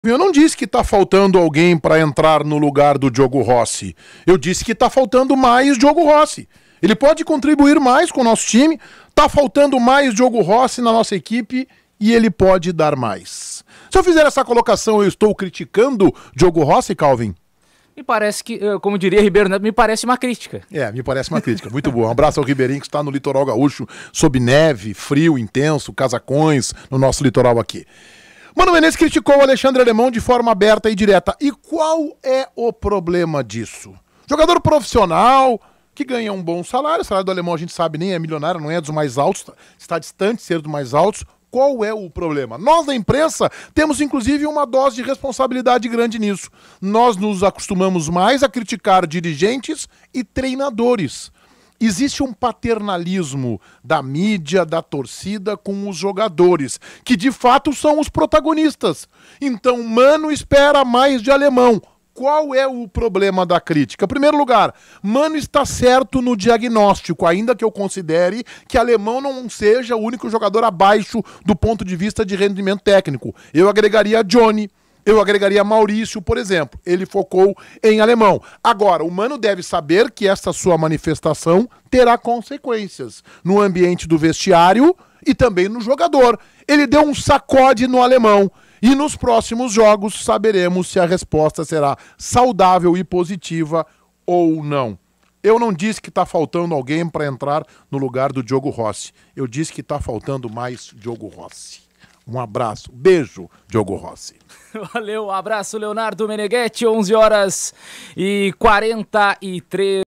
Mano Menezes critica Alemão, mas qual o problema? Confira o comentário de Leonardo Meneghetti
📳 Participe pelo BandZap: (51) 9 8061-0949
Segue a Band no Twitter e Instagram!